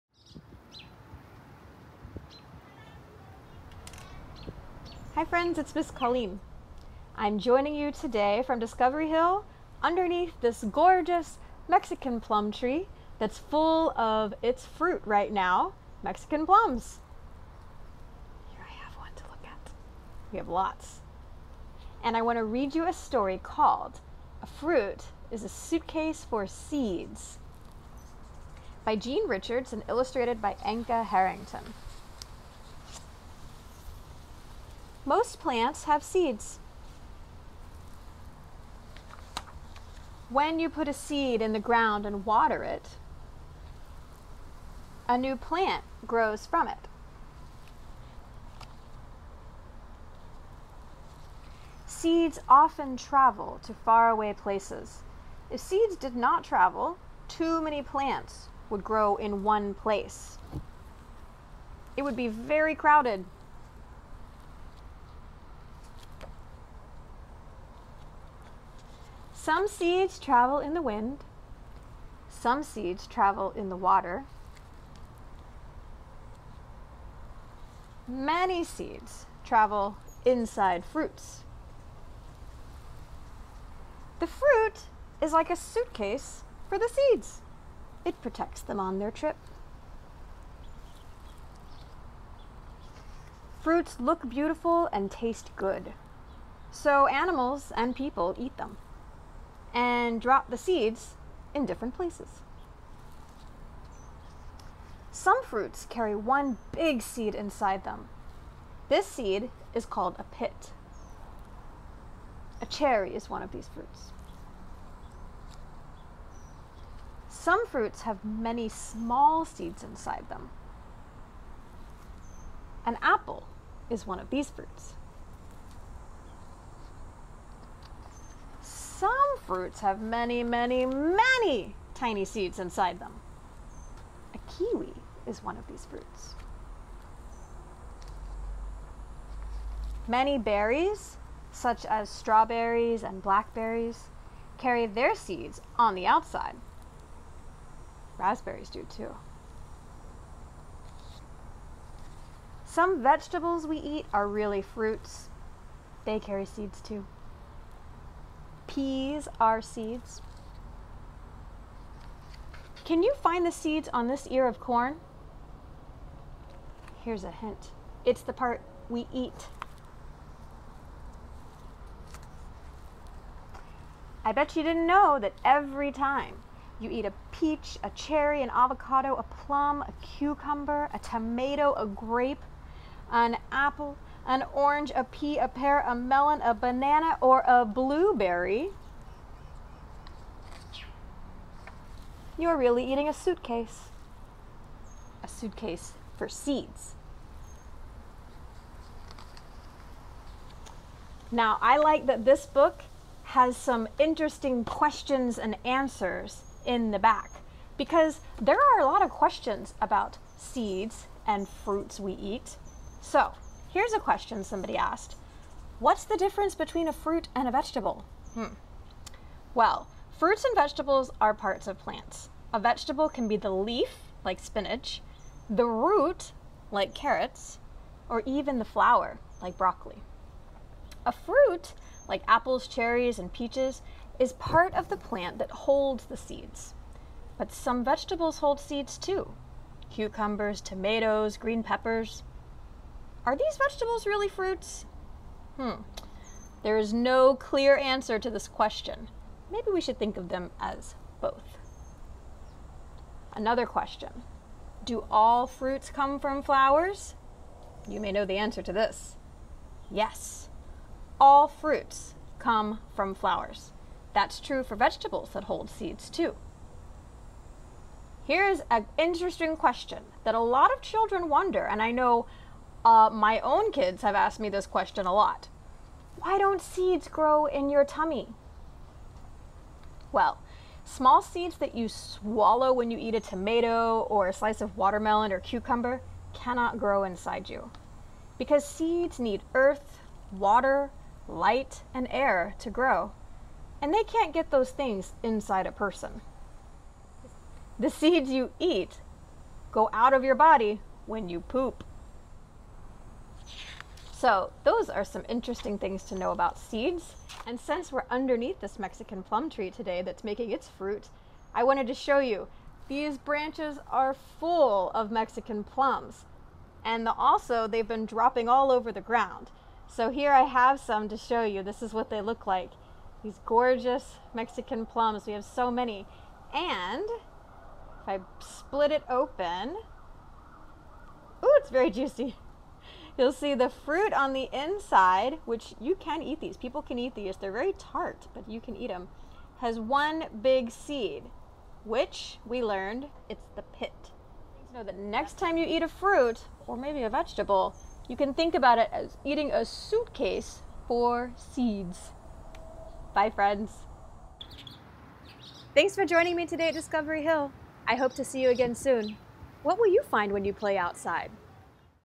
Story Time: A Fruit is a Suitcase for Seeds
A Fruit is a Suitcase for Seeds, read under a Mexican Plum tree at Discovery Hill Outdoor Learning Center. The book was written by Jean Richards, and illustrated by Anca Hariton.